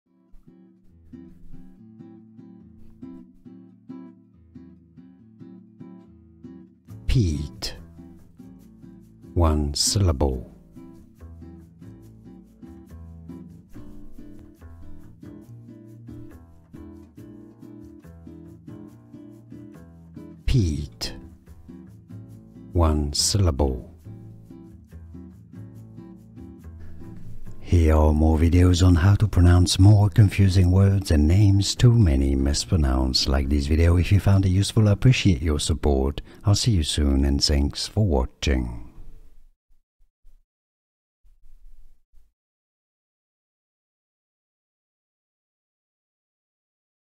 How to Pronounce Peat
This video shows you How to Pronounce Peat, pronunciation guide.

00:00 - Pronunciation Intro
00:06 - How to Pronounce Peat
00:28 - More Difficult Pronunciations